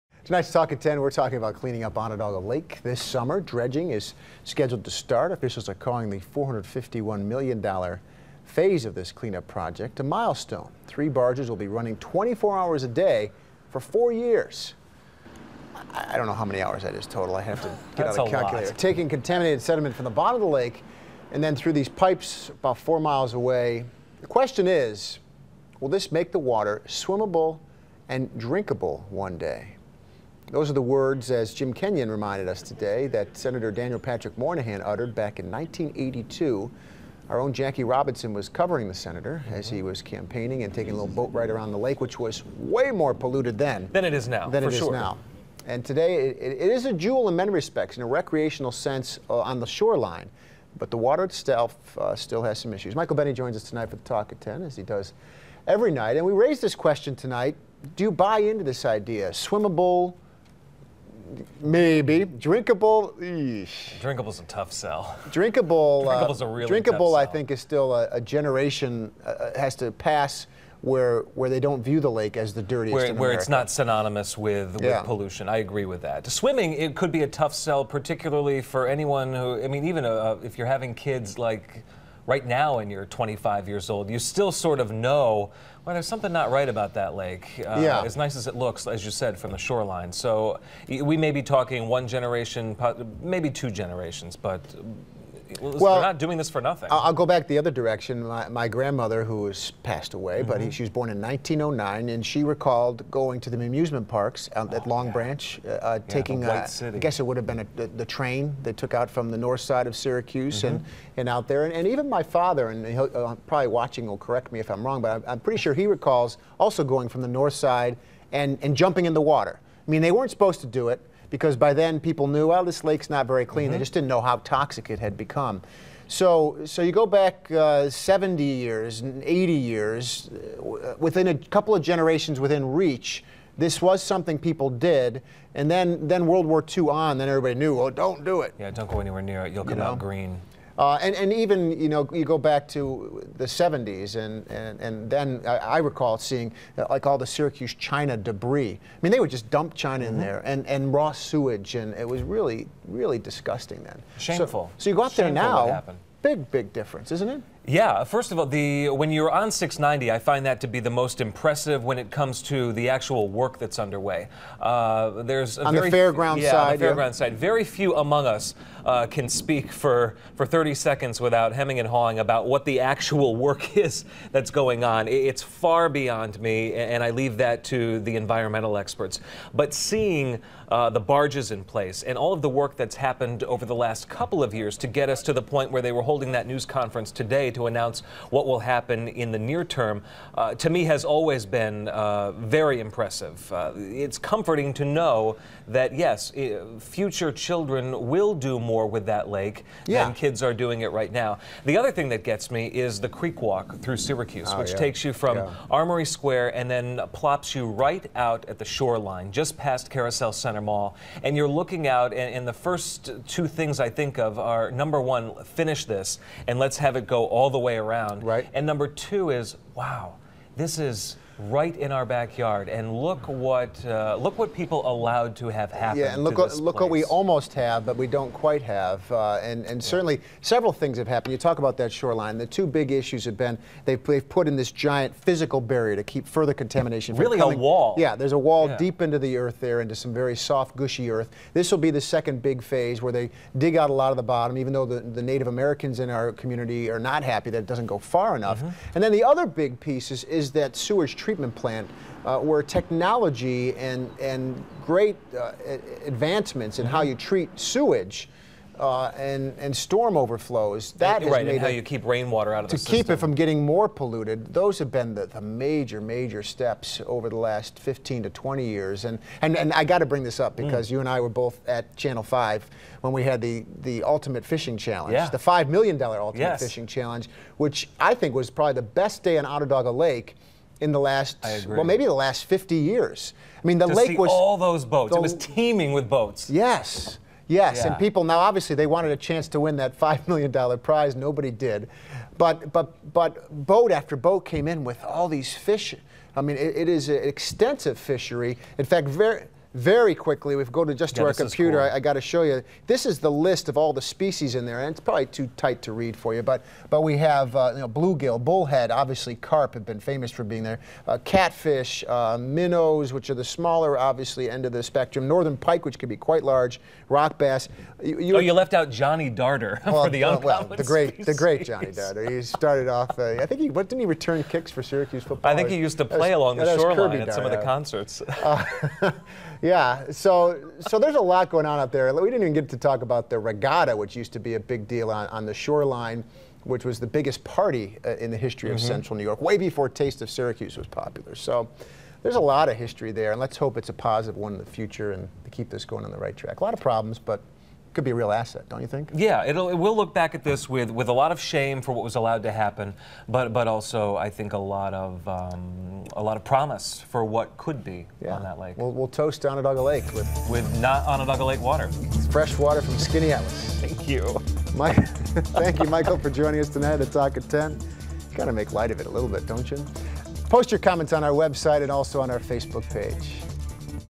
Talk at 10: Cleaning up Onondaga Lake
5-31-12

The cleanup of Onondaga Lake is about to meet a major milestone. Top state and federal officials are coming to the shoreline in Liverpool to "celebrate" it, according to a media advisory.

At 11:30 Thursday, State DEC Commissioner Joe Martens joined the head of the EPA's Superfund Program for New York, a Senior Vice President of the Honeywell Corporation, Mayor Stephanie Miner, the President of SUNY ESF and others.

The event is tied to the beginning of a four year dredging project that will commence later this summer.  Martens told the gather that Onondaga Lake "is making a huge comeback. It's going to be important for ecological reasons and economical reasons as well. It's just a win win for all."